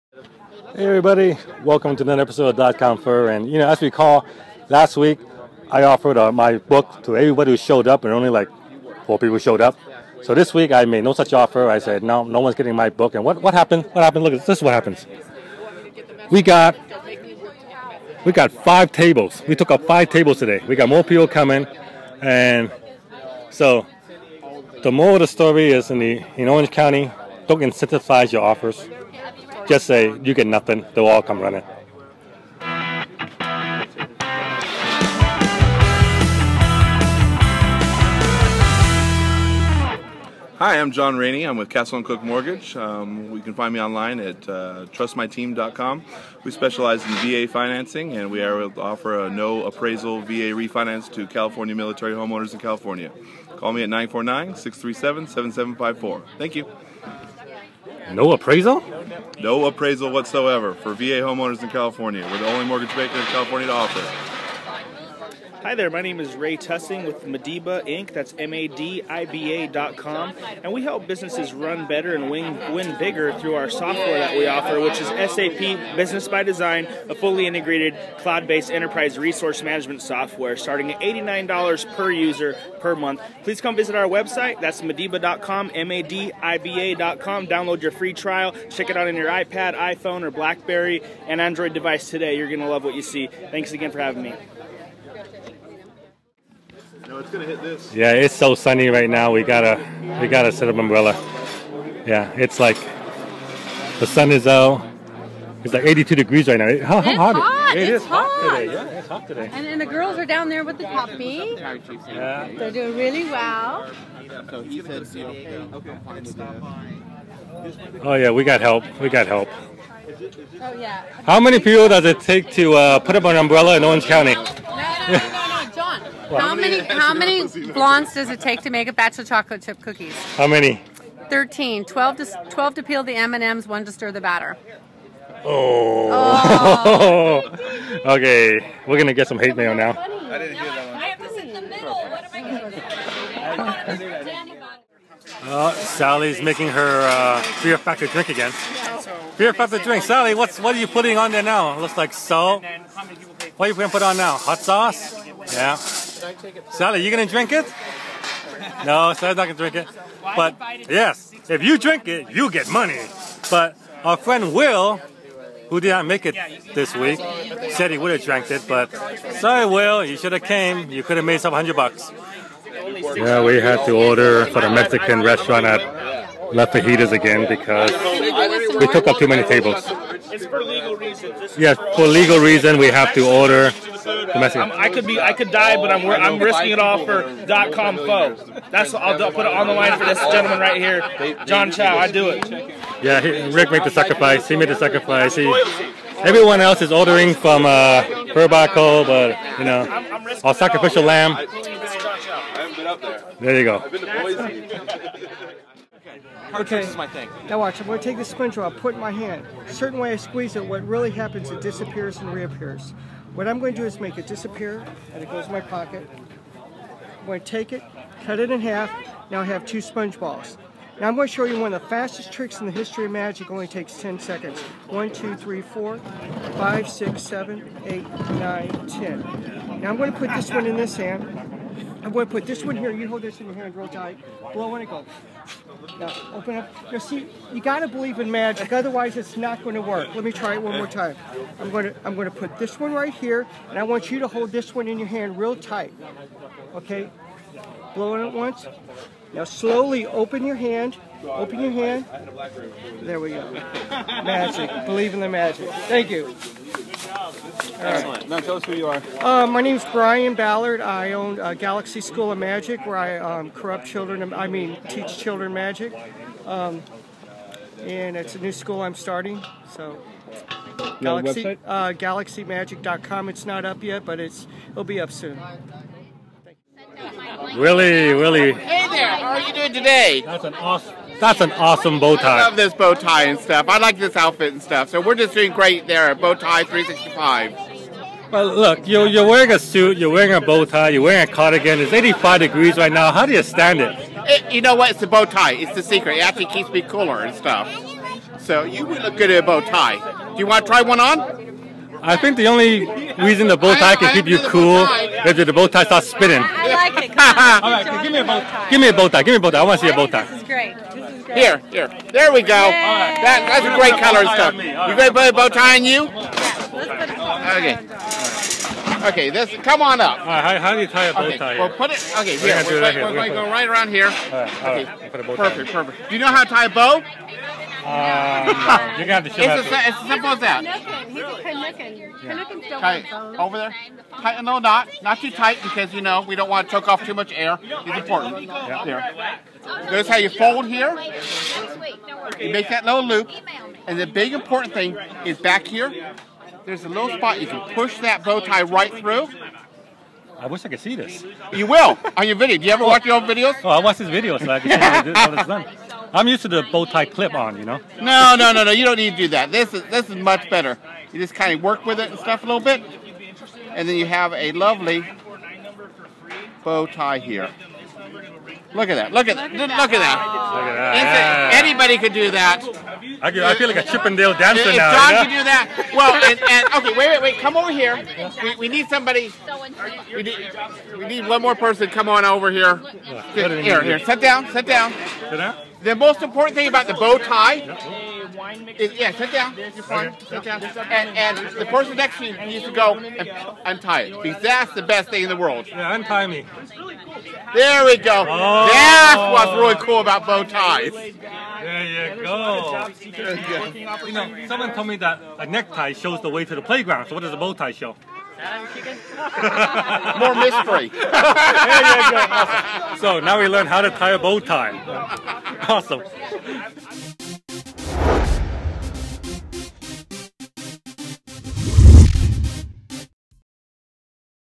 Dot Com Pho - How To Tie a Bow Tie Edition
We show how to tie a bow tie (a real bow tie, not a clip on) and a whole lot more.